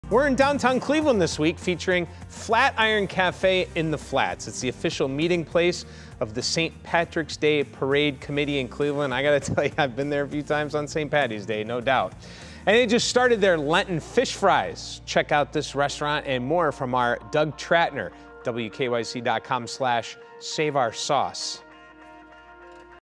Exploring the Flat Iron Cafe in downtown Cleveland
February 22, 2021: The Flat Iron Café keeps the spirit and vibe of the old Flats alive and well in the midst of great change. The popular Irish watering hole has been a Cleveland staple since the early 1900s, and the warm space still merits praise. While the bar is the main draw for many, rib-sticking comfort food like pierogis, meat loaf and mashed potatoes, corned beef and cabbage and Friday fish fries have their own fans.

This feature is part of the 3News 'Save our Sauce' campaign, which is being done to highlight Northeast Ohio restaurants amid the COVID-19 pandemic